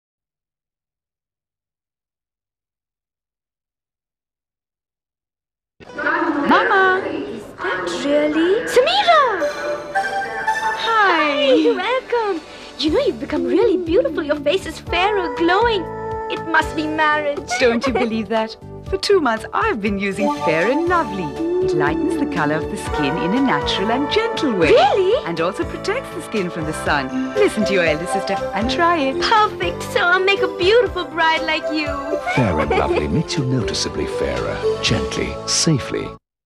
Fair and lovely cream advertisement - Mahima Chaudhry - Miss india Aishwarya rai 1994
Aishwarya appeared in the ad for the famous skin-lightening cream Fair & Lovely with another actress Mahima Chaudhry. The two are shown as sisters in the ad and Aishwarya shares that she has been using the cream which lightens the colour of the skin in a natural and gentle way.